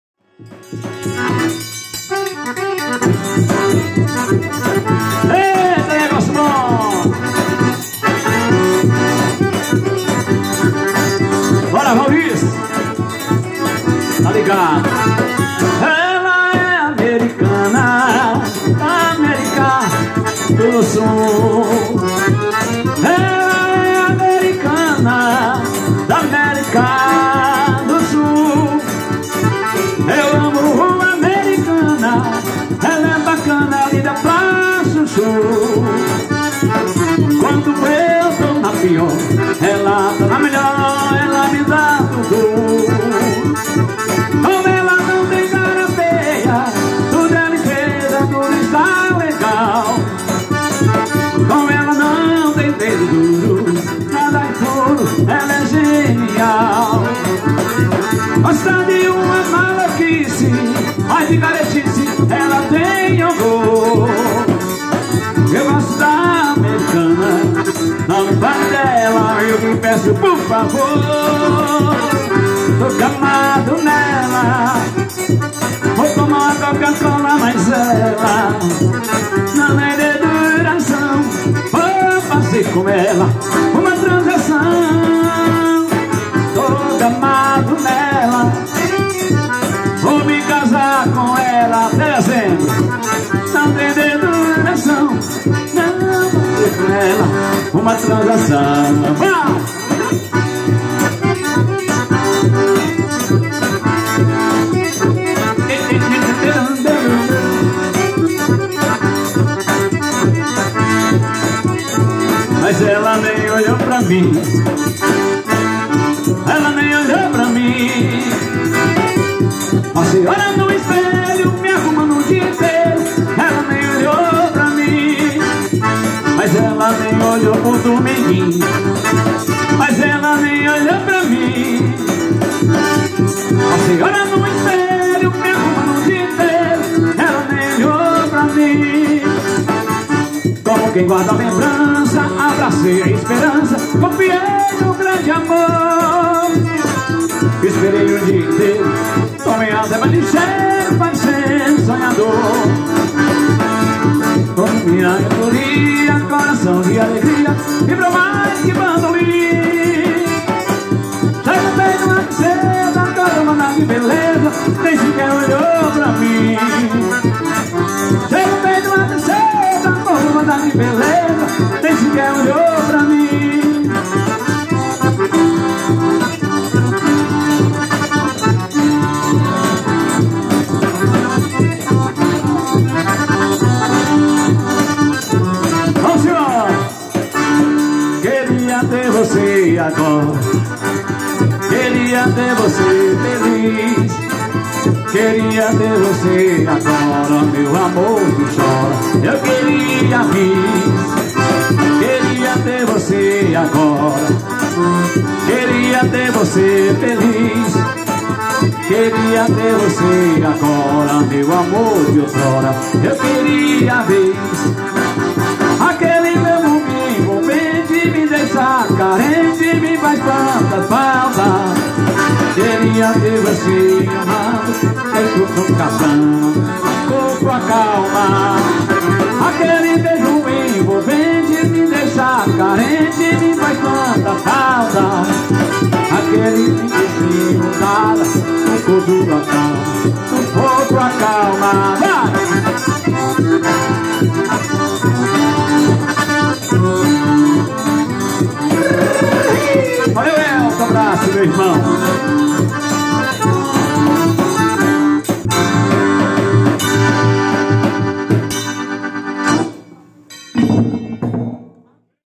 Trio do São João: Deusdete Morais, Neto do Estrela do Forró e Chaguinha do Zabumba.
Trio do São João: Deusdete Morais, Neto do Estrela do Forró e Chaguinha do Zabumba no encerramento do tríduo de São João em Coivaras Piauí.